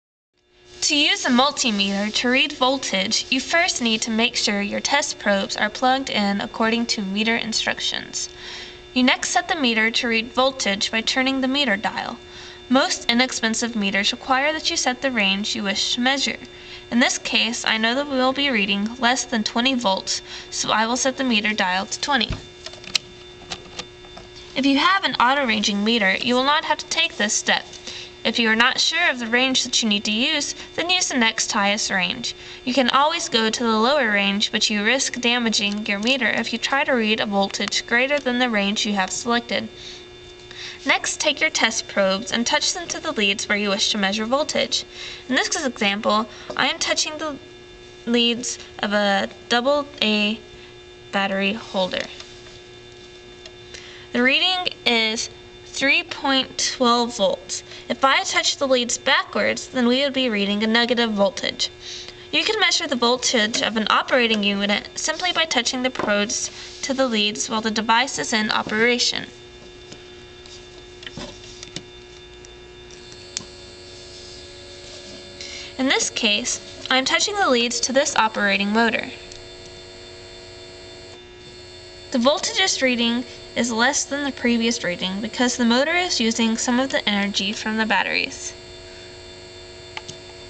Reading Volts with a multimeter
How to read volts using a multimeter.  This is a instructional video on the technical reference site DI Tech DIcoded at tech.texasdi.org